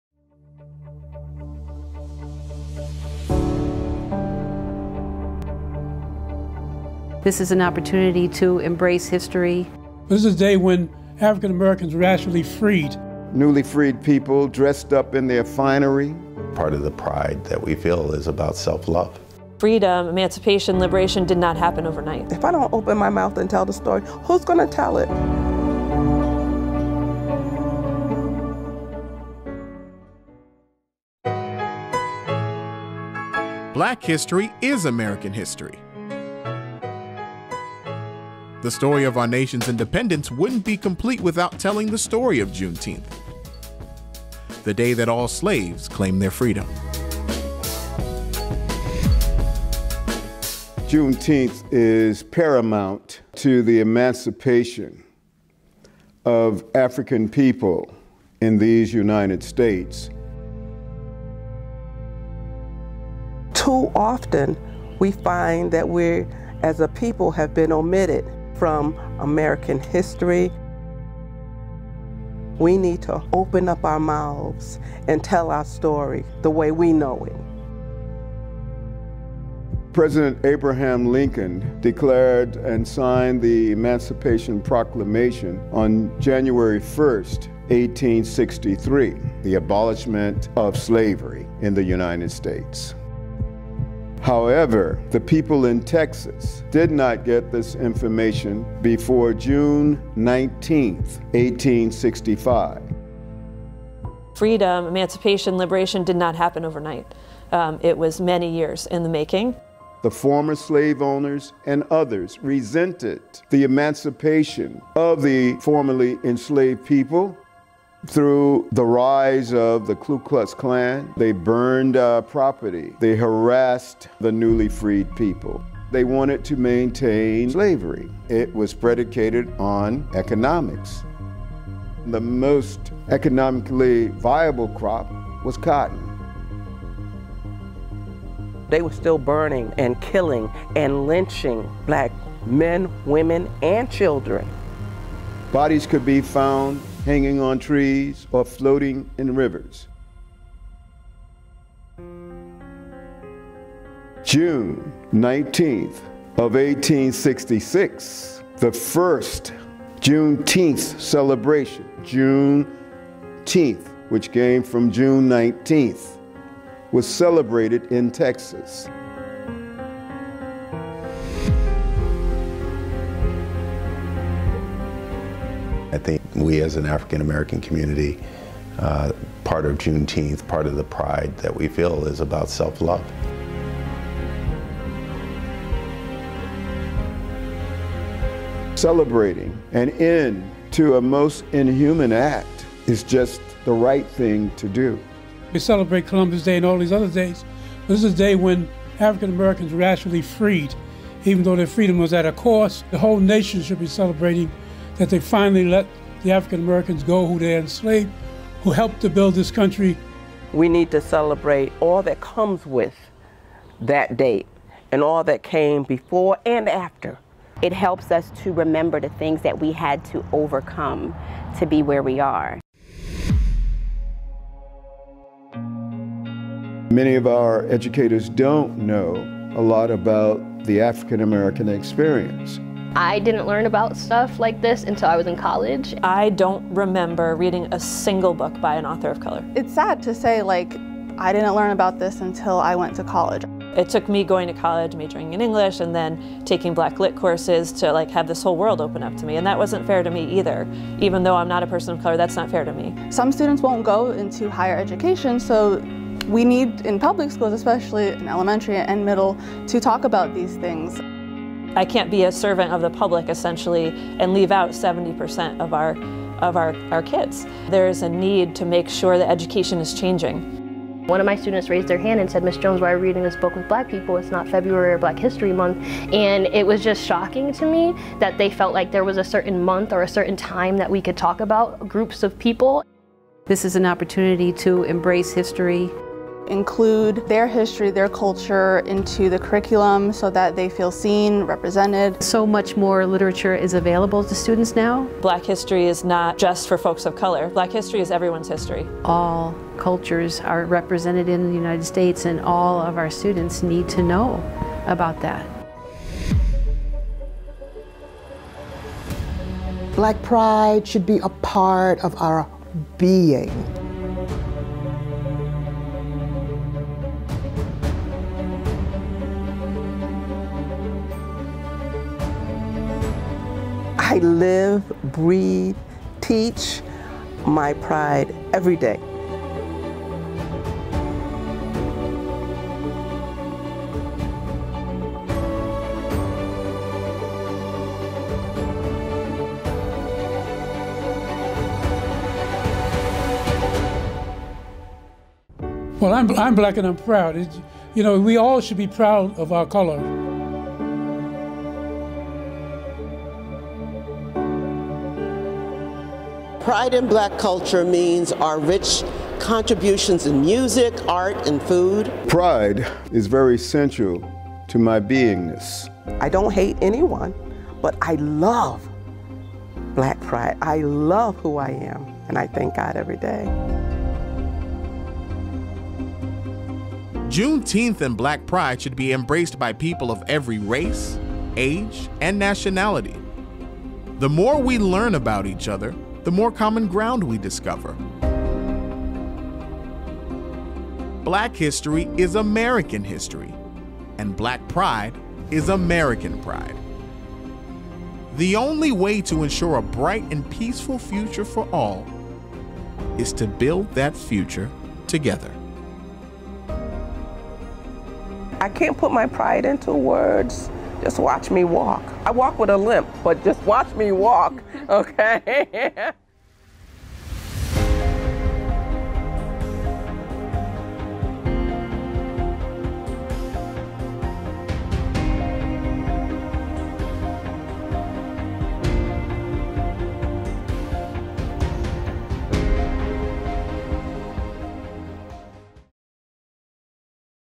A story of Juneteenth and its importance in America today
A celebration of Juneteenth begins with a telling of its history.